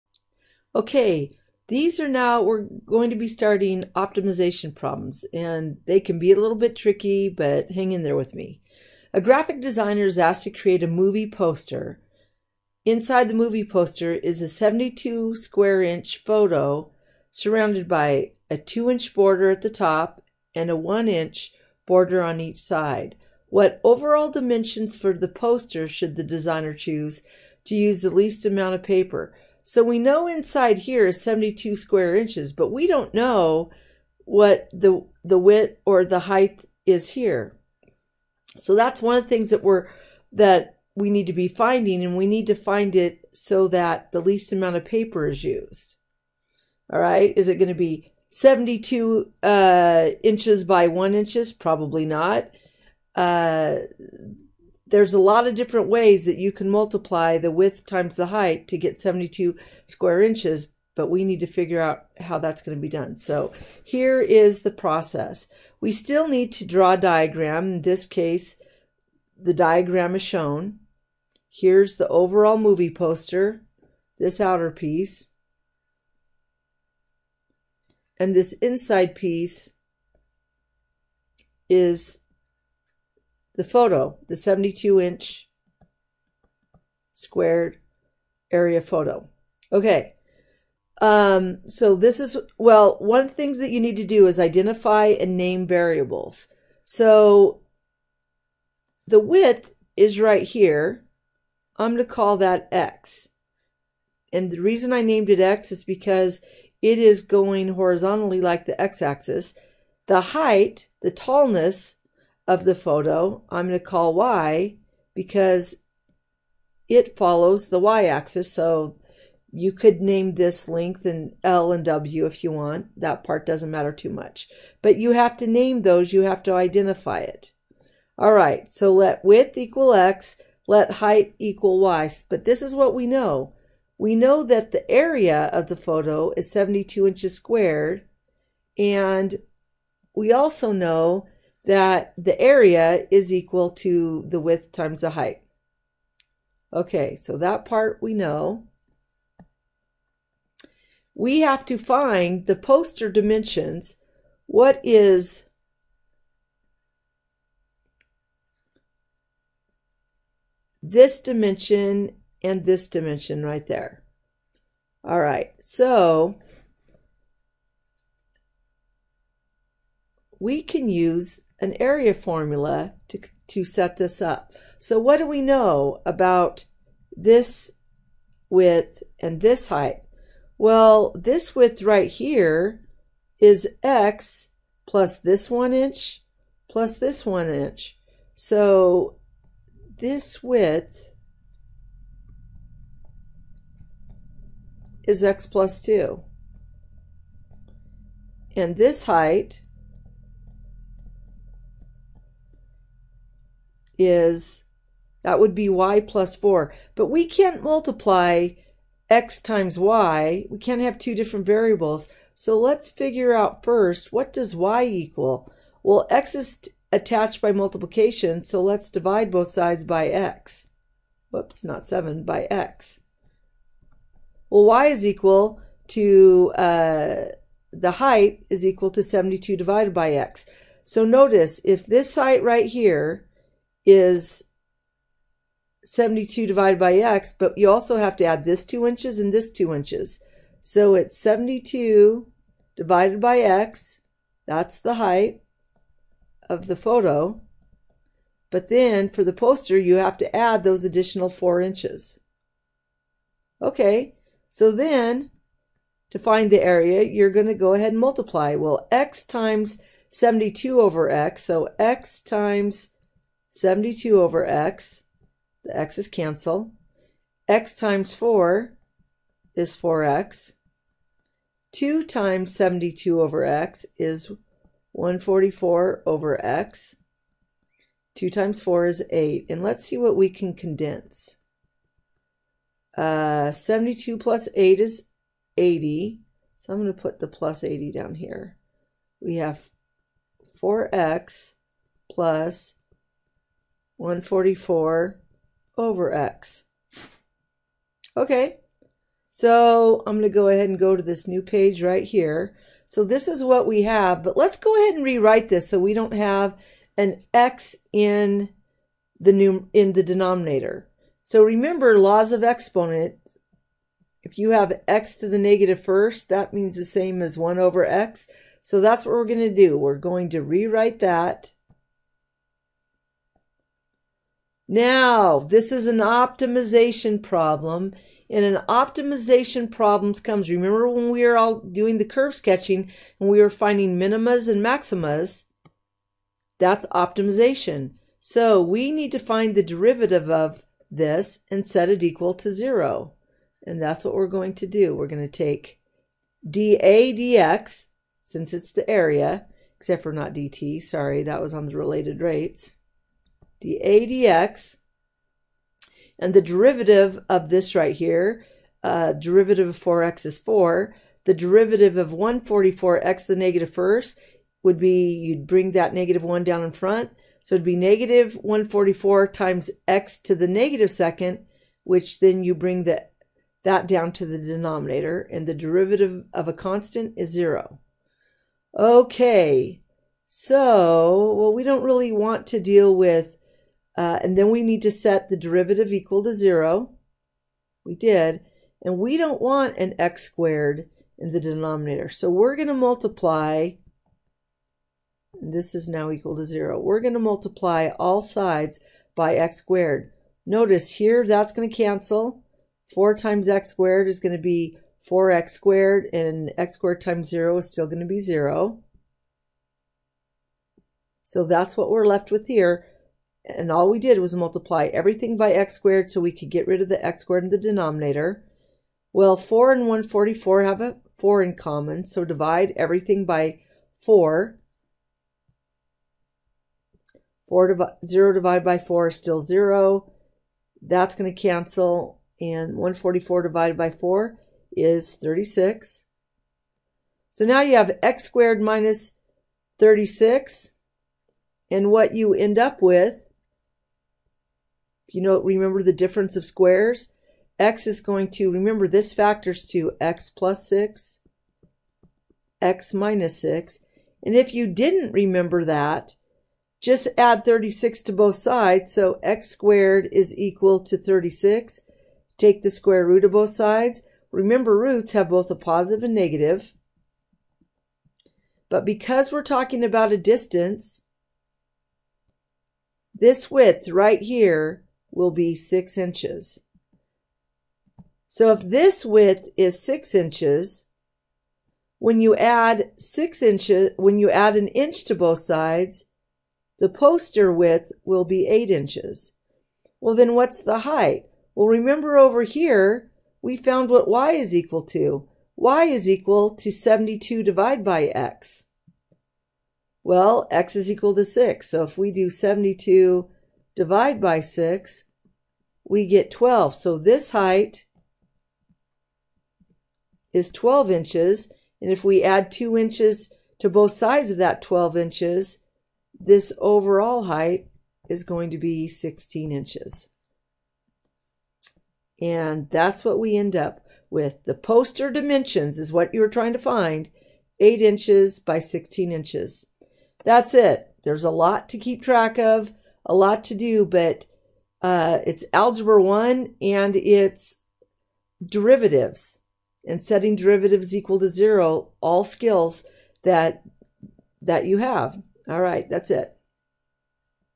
Optimization: Rectangle (Poster Size)
This video shows the steps for optimization problems when the task to minimize the size of a poster.